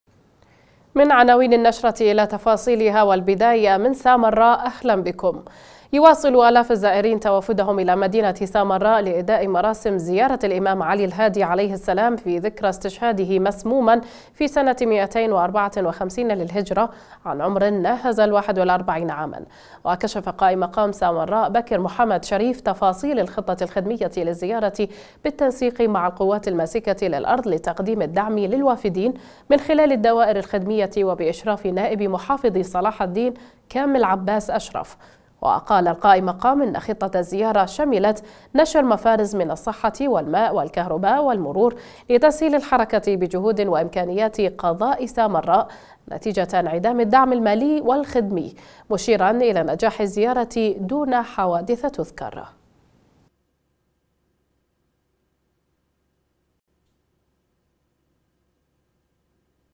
الاف الزائرين يحيون مراسيم سامراء بذكرى استشهاد الأمام الهادي
يواصل آلاف الزائرين توافدهم إلى مدينة سامراء لأداء مراسيم زيارة الإمام علي الهادي عليه السلام في ذكرى استشهاده مسموما في سنة مئتين وأربعة وخمسين هجرية عن عمر يناهز الواحد والأربعين عاما، وكشف قائمقام سامراء بكر محمد شريف تفاصيل الخطة الخدمية للزيارة بالتنسيق مع القوات الماسكة للأرض لتقديم الدعم للوافدين من خلال الدوائرالخدمية وبإشراف نائب محافظ صلاح الدين كامل عباس اشرف، وقال القائممقام: ان خطة الزيارة شملت نشر مفارز من الصحة والماء والكهرباء والمرور لتسهيل الحركة بجهود وإمكانيات قضاء سامراء نتيجة انعدام الدعم المالي الخدمي، مشيرا الى نجاح الزيارة دون حوادث تُذكر.